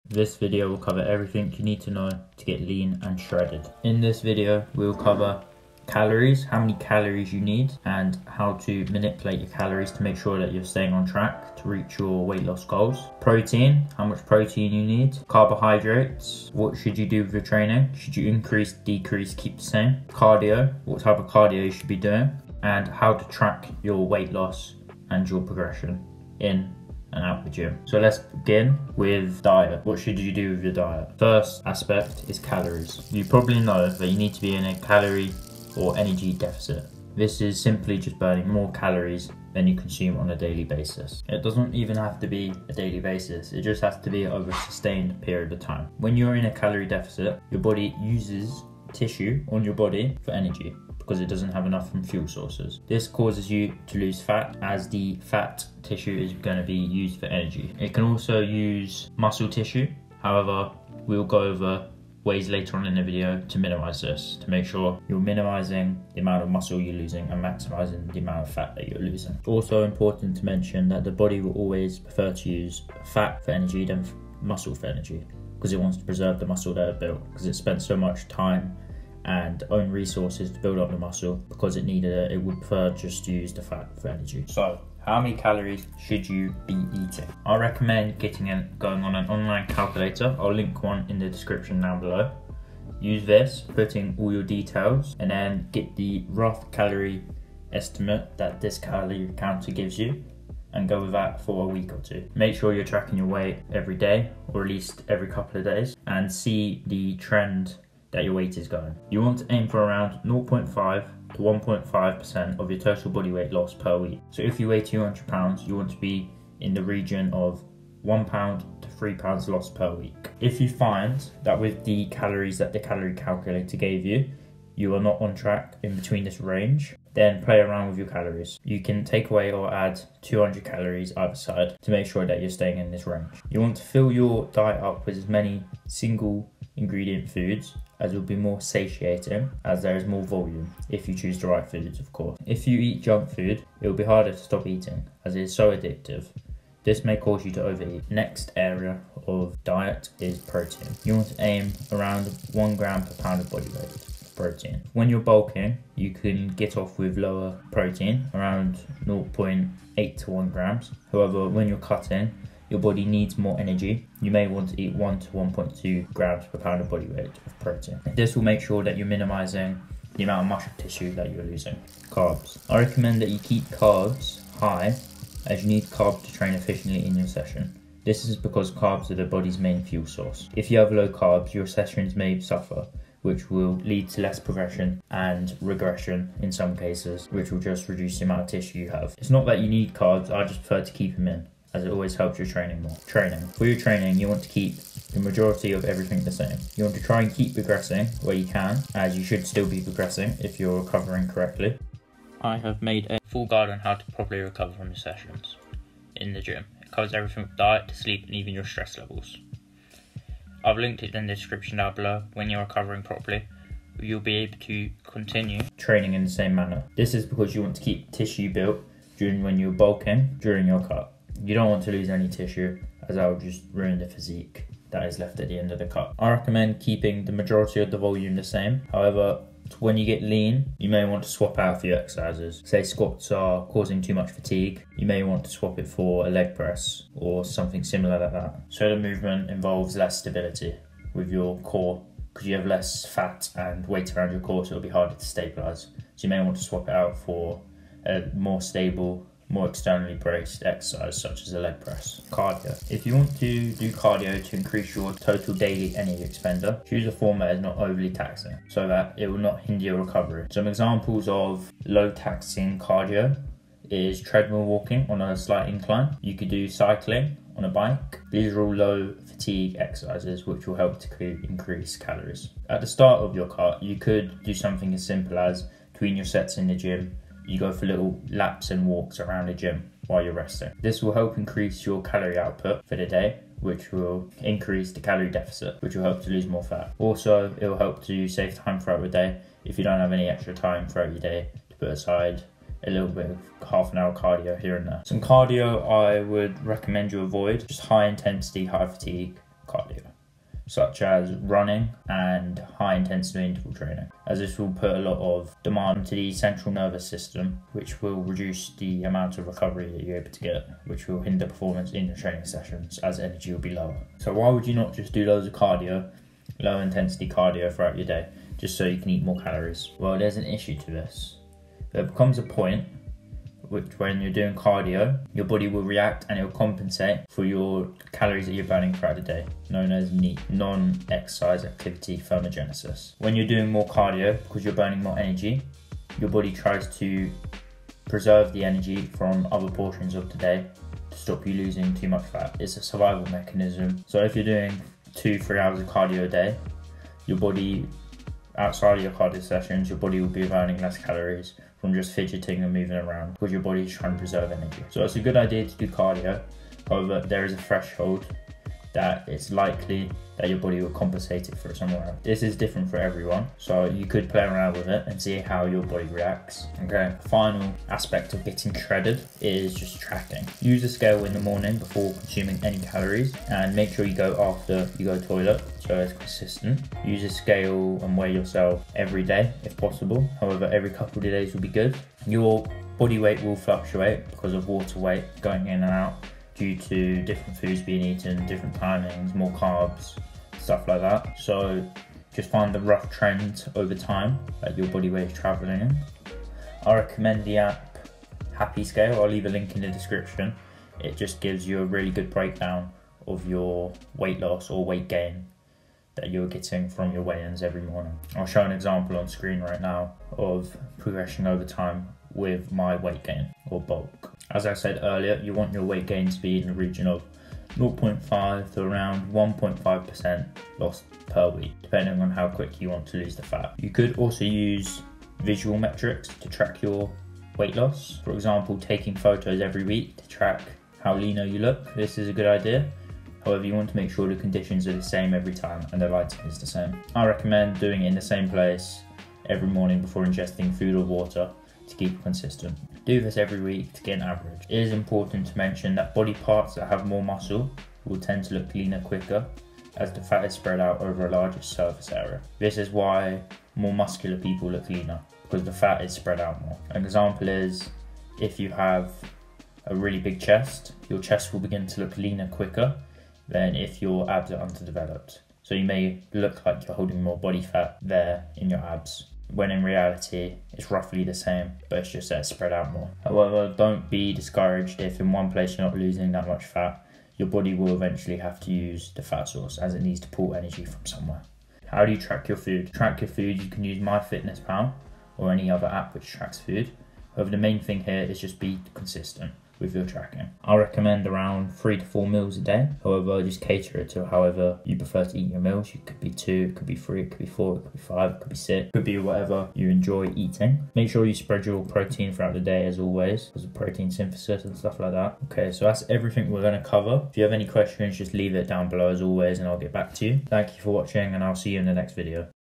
How To Get and Stay Shredded
This video is on how to get shredded and lean. You’ll learn how how many calories, how much cardio if any, what to do with training, how to track metrics and much more. Enjoy

TIMESTAMPS

0:00 INTRO
0:30 CALORIES
2:30 PROTEIN
2:50 CARBS
3:13 TRAINING
4:10 CARDIO
6:10 TRACKING